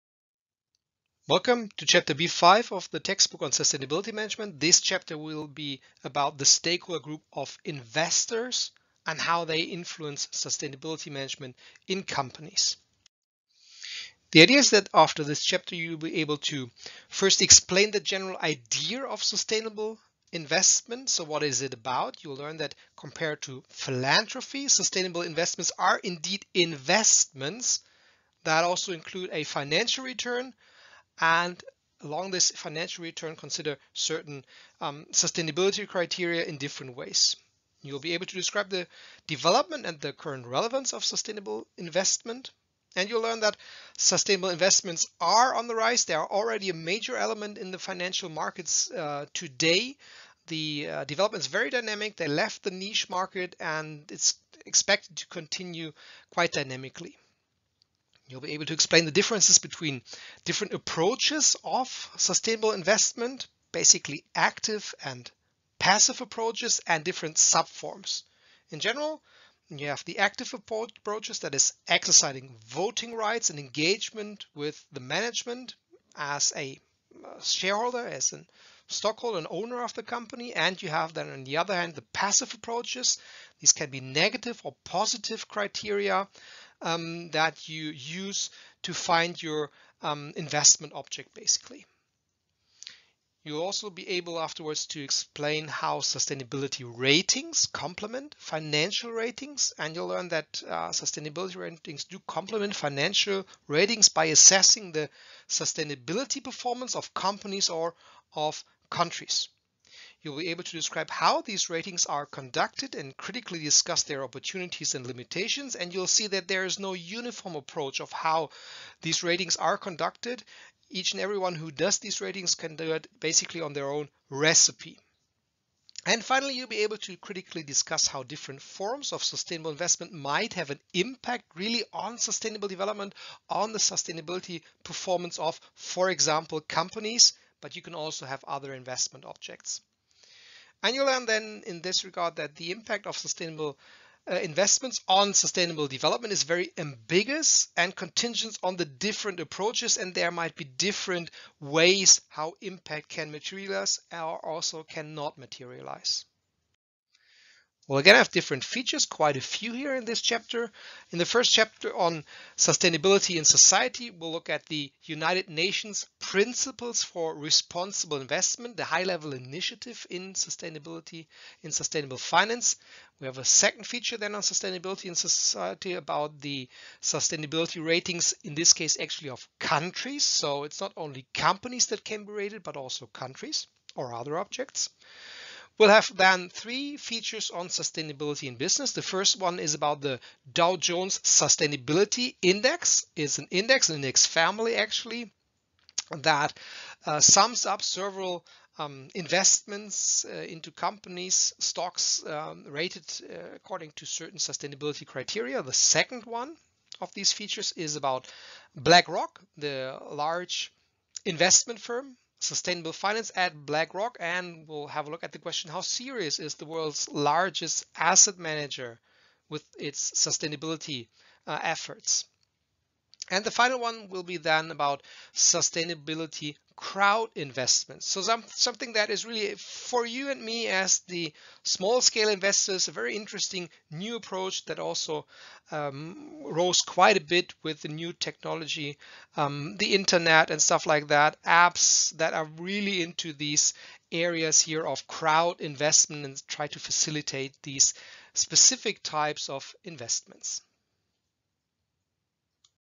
Sustainability Management: Investors and Sustainable Finance
Introductory Screencast on "Investors". Chapter B.5. of the textbook "Sustainability Management - Global Perspectives on Concepts, Instruments, and Stakeholders"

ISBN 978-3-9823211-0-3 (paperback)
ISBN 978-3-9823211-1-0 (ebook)